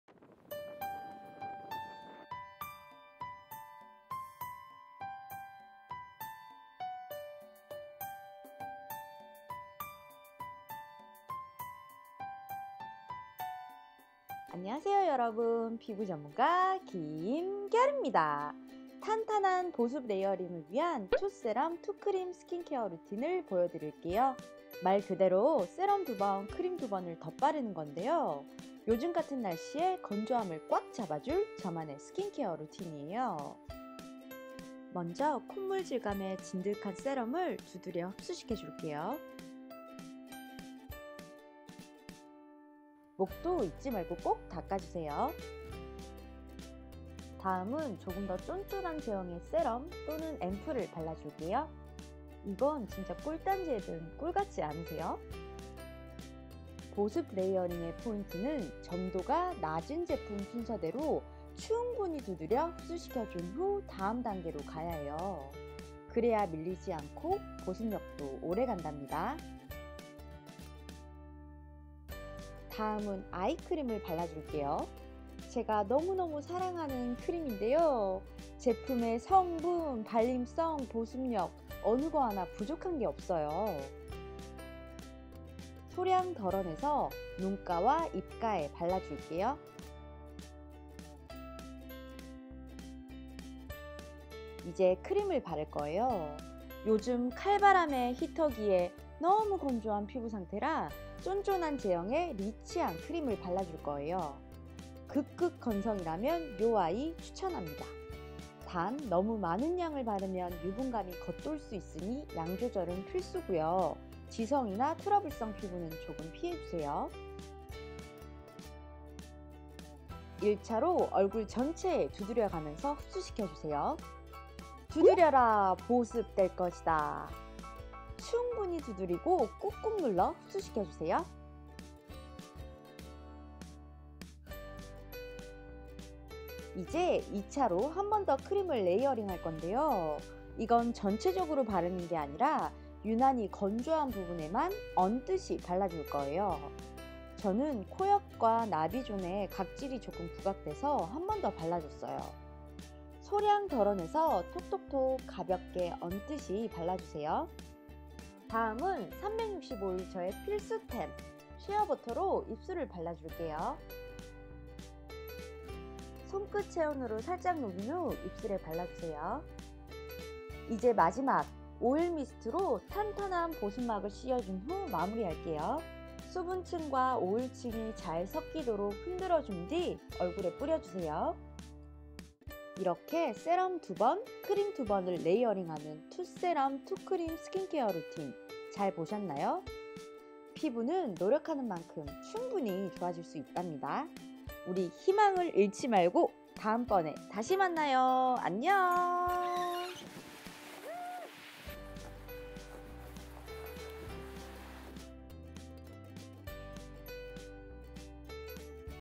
【월간루틴】2月ㅣ보습 레이어링 스킨케어 루틴 | KOREA FEBRUARY SKINCARE ROUTINE | 2017
안녕하세요. 피부전문가 김결입니다!
오늘은 건조하고 너무 너~~~무 추운 2월의 스킨케어 루틴을 가지고 왔습니다.

추운 겨울에 옷을 껴입어 체온을 유지하고 몸을 보호하듯,
우리 피부도 수분을 빼앗기지 않게 ‘수분 레이어링’을 해주는게 정말 중요해요.

그래서 오늘은 7스킨법에 이어 대유행 할(?) '2세럼2크림'을 가지고 왔답니다.
이건 비밀인데 ‘2세럼2스킨’은 제가 최초로! 알려드리는거예요.
왜냐면 제가 만든거니까요. 하하핫

그리고 맨날 일찍 영상 가지고 올 것처럼 해놓고 늦게 와서 미안해요 ㅜㅜ
그래도 항상 이쁘게 봐주시는 구독자님들 싸랑하고 늘 고마워요~❤❤❤

[제품 상세설명]

1.아로마티카 / 로즈 앱솔루트 퍼스트 세럼 130ml 31,000원

2.싸이닉/ 허니 올인원 앰플 250ml 25,000원

3.마티나 겝 하르트/ 아보카도 링클 아이 크림 15ml 15,000원

4.리놀라/ 페트 50g 25,000원

5. 록시땅/ 퓨어 시어 버터 150ml 55,000원

6.어썸/ 하이드레이팅 더블 미스트 120ml 22,000원

저의 일상이 궁금하시다면 !

피부고민이나 궁금하신 점은 댓글로 꼭 남겨주세요. :)